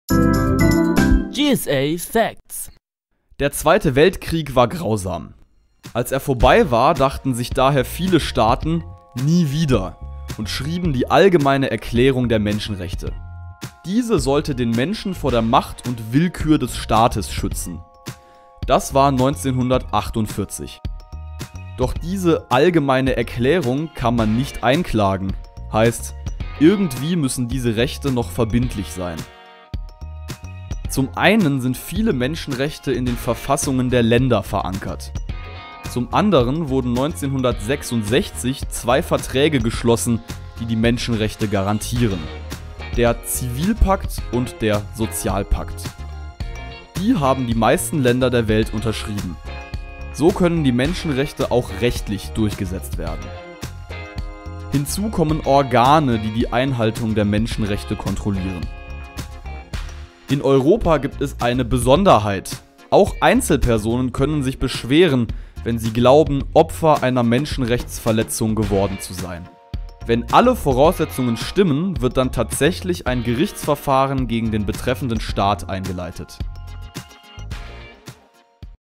Menschenrechte: Erklärvideo (GSA Facts)
Viel wird über sie geredet, häufig wie selbstverständlich. Viele halten sie für absolut richtig und extrem wichtig, dabei werden sie jederzeit auf der ganzen Welt verletzt. Es geht um die Menschenrechte.
In diesem GSA-Facts zeigen wir die Entstehung und die rechtliche Durchsetzung der Menschenrechte auf.
Es sei euch ein Bildungszuwachs!

ein Beitrag von Valentin Heib.
gesprochen von Valentin Heib.
Video/Animation von Nicolai Boldt.

©Geschwister-Scholl-Antenne 2020
Bildquellen: